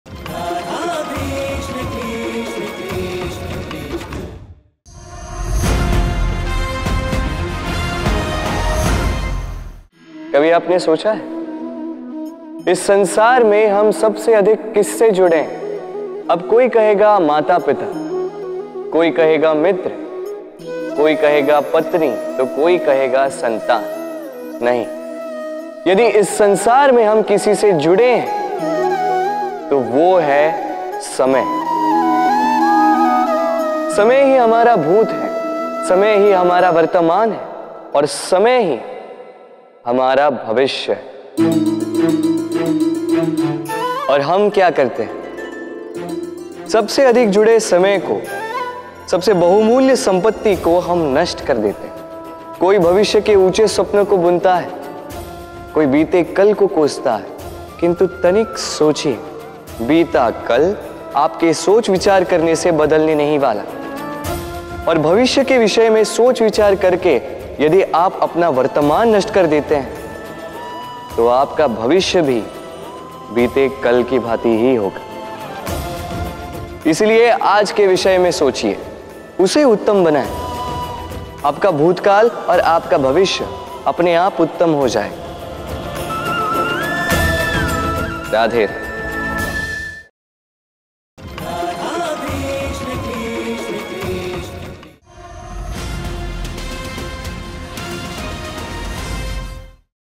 Krishna-vani | Samay sabse bahumoolya hai | कृष्णवाणी Part-123
►*About*
Samay humaare jeevan mein sabse bahumoolya hai. Samay hi bhoot, vartmaan aur bhavishya hai, lekin hum iss samay ko hi sabse adhik nasht karte hain, kabhi bhavishya ki chinta, toh kabhi bhoot ka pachhtaava. Yadi hum apne vartamaan ko sanwaar lein toh bhoot aur bhavishya apne aap hi sanwar jaayenge, kyunki jo aaj vartamaan hai wahi kal bhoot hoga aur vartmaan hi bhavishya ki neenv hai.

कृष्ण वाणी - Part - 123
 Samay sabse bahumoolya hai